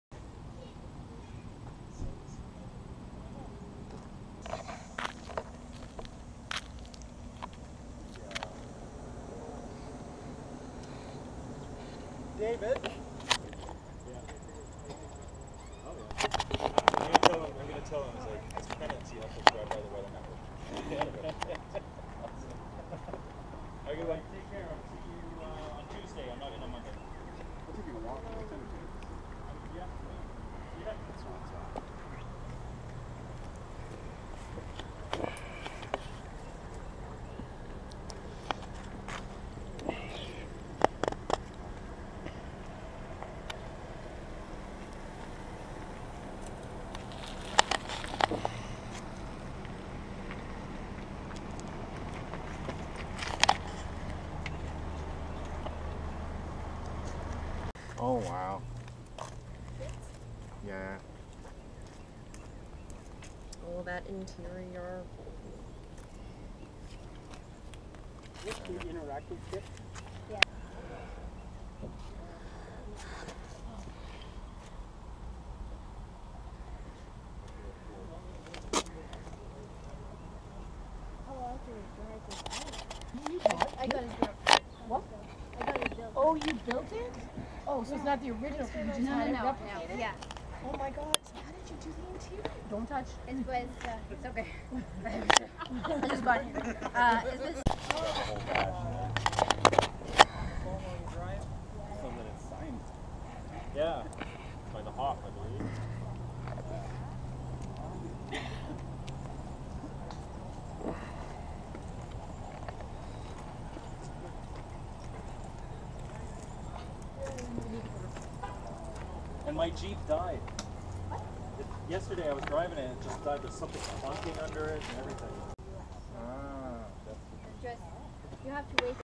Knight Rider at props expo
a few random shot of classic 80's tv show.